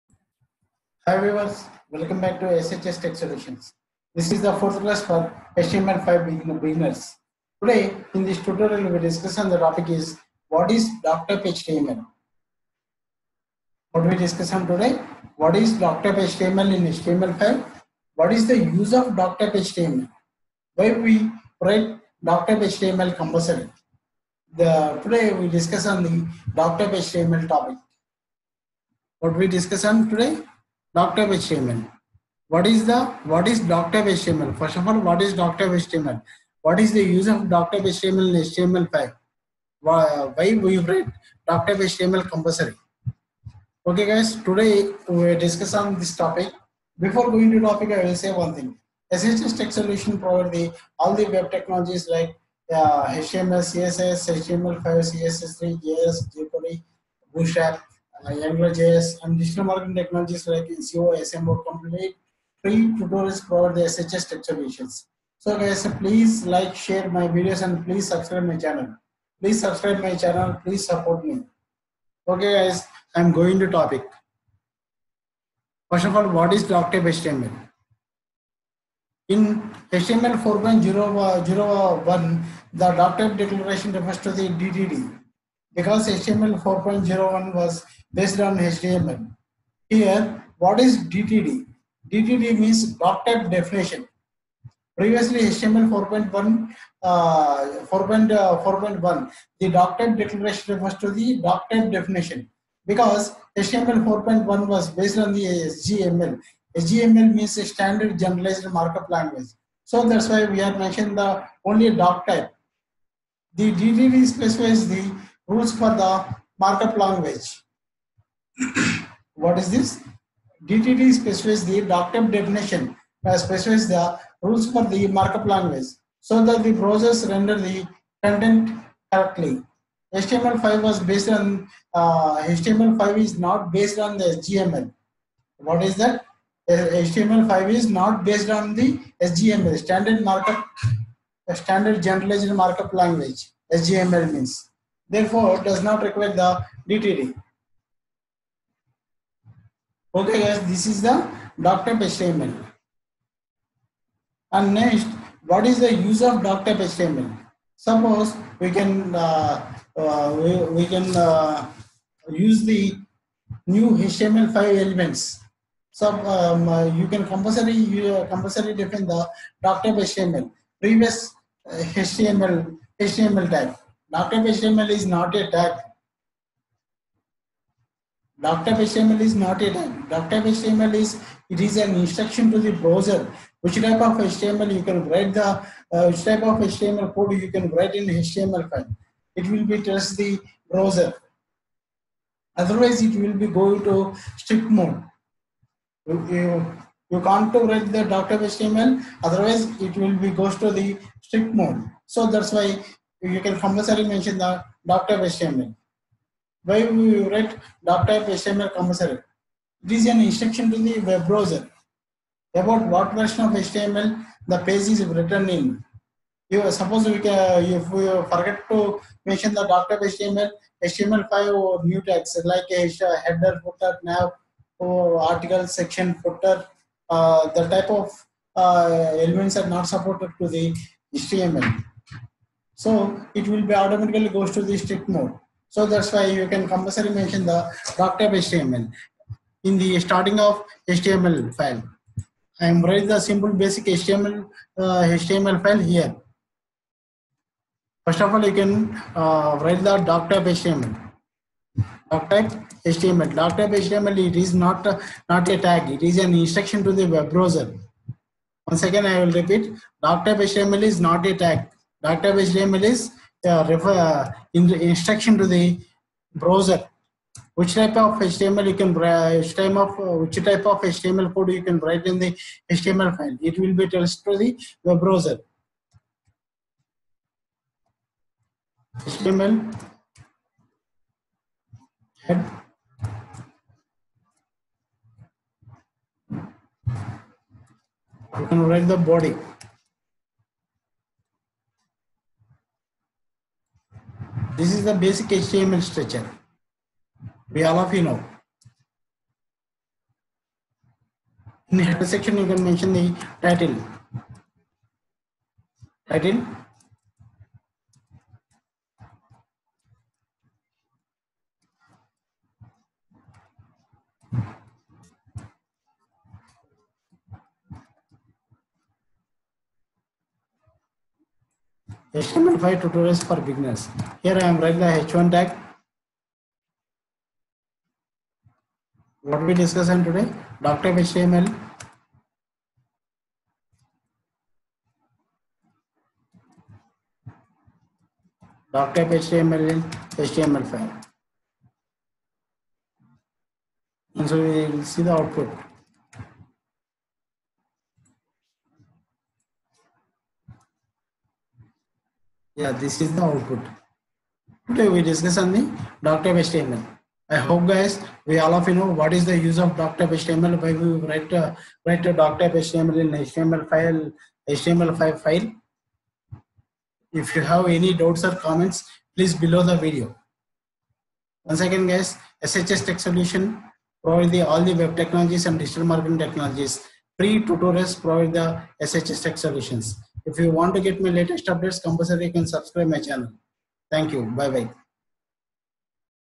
Html5 Tutorial For Beginners - 04 | DOCTYPE Html | DOCTYPE Declaration In Html
Html5 Tutorials For Beginners – 04 - DOCTYPE html
Today in this tutorial I was discuss on the topic is DOCTYPE html
1)  What is DOCTYPE html ?
2)  What is the use of DOCTYPE html in html5 ?
3)  Why we write DOCTYPE html compulsory ?
1Ans) In HTML 4.01, the !DOCTYPE declaration refers to a DTD, because HTML 4.01 was based on SGML.
 The DTD specifies the rules for the markup language, so that the browsers render the content correctly. HTML5 is not based on SGML, and therefore does not require a reference to a DTD.

3Ans)   It is an instruction to the web browser about what version of HTML the page is written in.
If we forget to declared doctype then many new features & tags in HTML5 such as article,footer,  header may not be supported .
Additionally, the browser may go automatically into Quirks or Strict Mode.